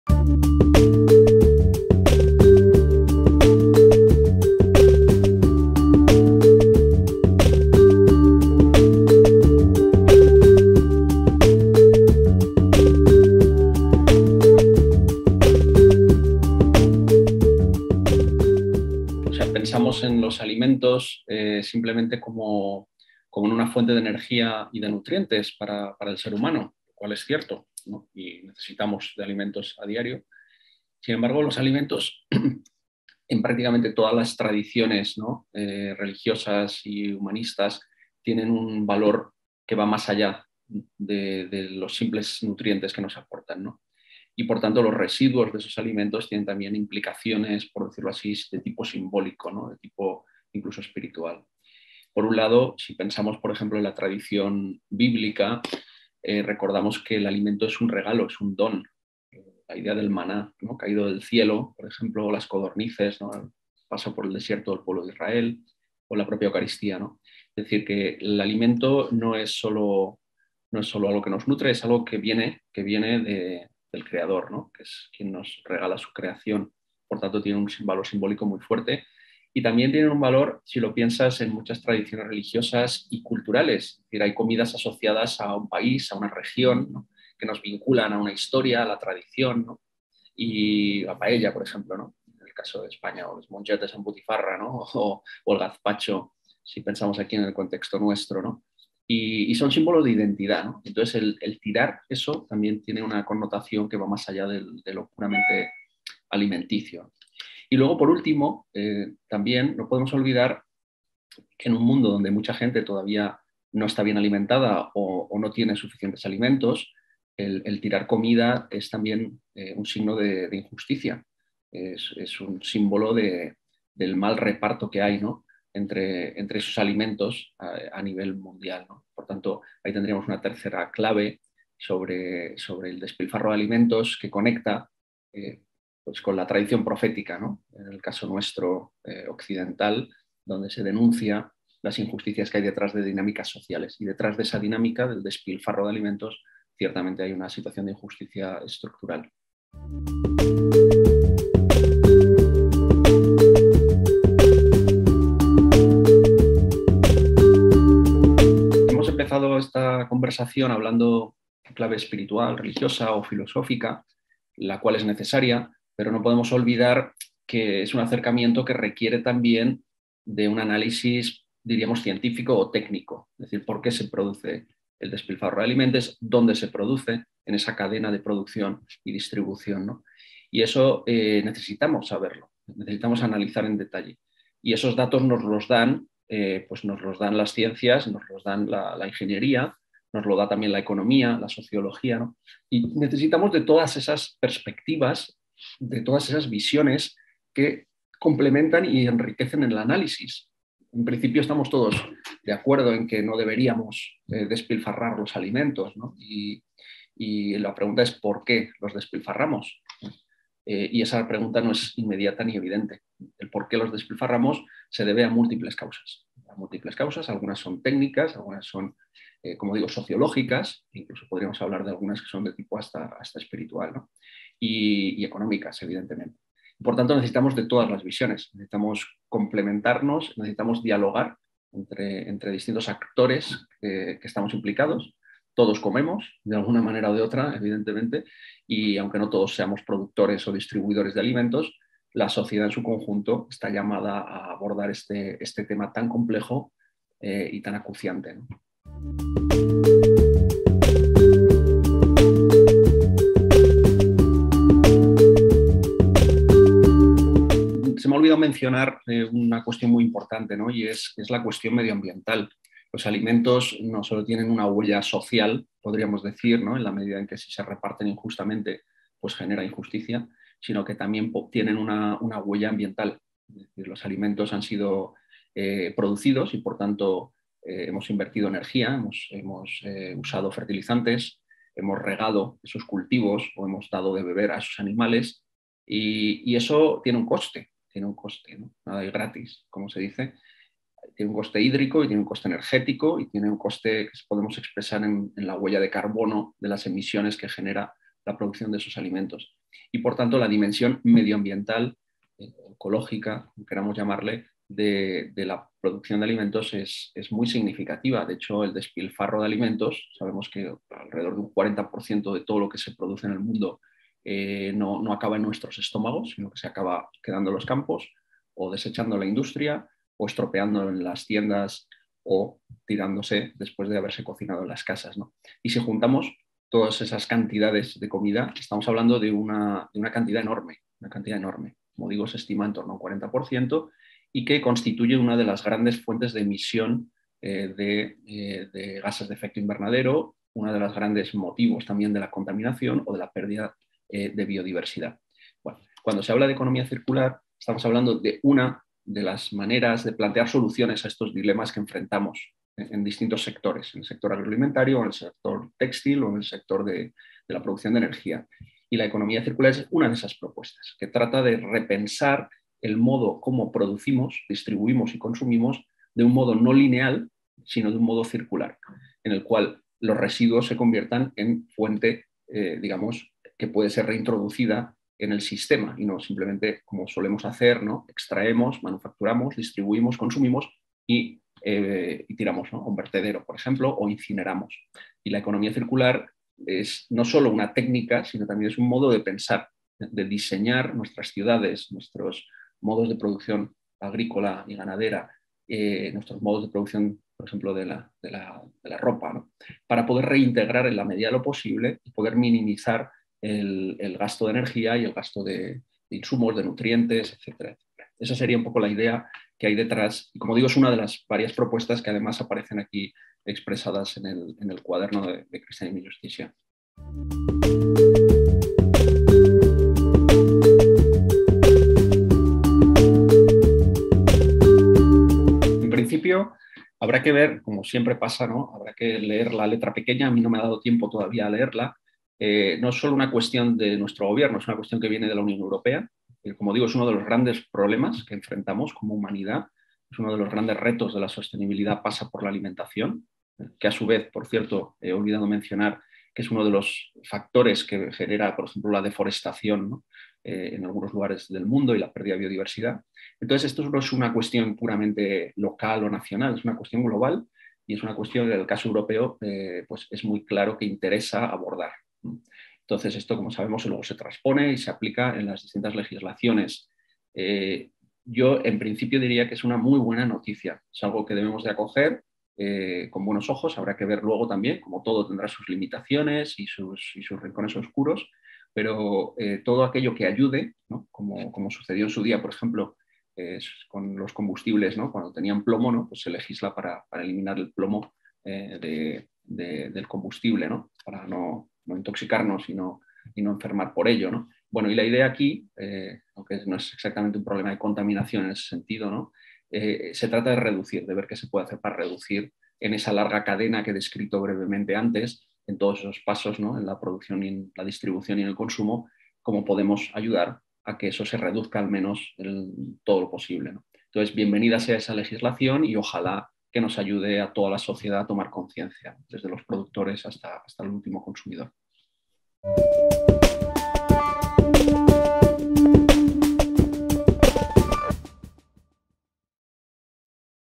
Entrevista con Jaime Tatay: «El desperdicio de alimentos»
En esta entrevista, el jesuita y experto en ecología, Jaime Tatay, presenta el Cuaderno CJ n. 228, «El desperdicio de alimentos». Los siete autores del cuaderno pertenecen al proyecto Higher Education for Social Transformation (HEST), promovido por la Conferencia de Provinciales Jesuitas de Europa (JCEP) y la red Kircher Network.

"El viaje de los alimentos hasta nuestra mesa y hasta el cubo de la basura (si no los comemos), ya no transcurre de manera aislada. Hemos de entender que nuestra relación con la comida y el desperdicio de alimentos afecta la vida de las personas en nuestro planeta tanto las que pasan hambre como las que envenenamos con lo que lanzamos. Es, por esta razón, que desperdiciar alimentos adquiere la categoría de sacrilegio y el encontrar solución se convierte en una cuestión que afecta no solo a la economía y a la salud sino también a la vida religiosa y espiritual. Así nos lo explica este cuaderno. Los autores son miembros del proyecto Higher Education for Social Transformation (HEST), promovido por la Conferencia de provinciales jesuitas de Europa (JCEP) y la red Kircher Network".

Cristianisme i Justícia se sostiene gracias a las aportaciones de personas como tú.